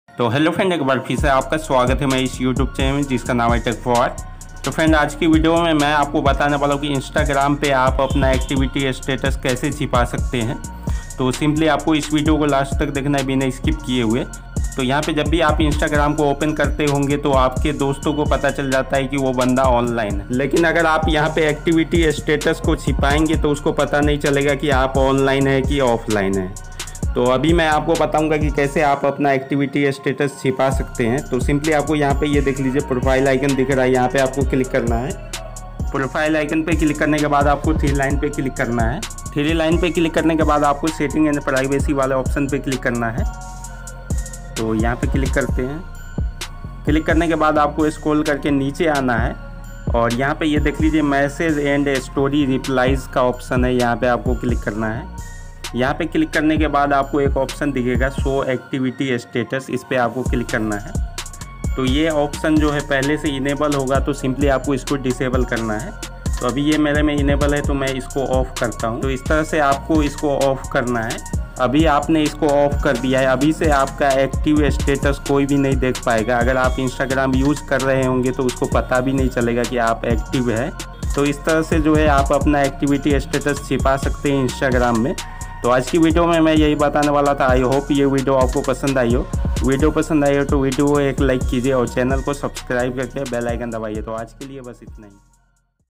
instagram me online status kaise chhipaye / Instagram me online hokar bhi offline kaise dikhe, Insta
instagram me online hokar bhi offline dikhne ka tarika sikhe
instagram me apna active status chhipana sikhe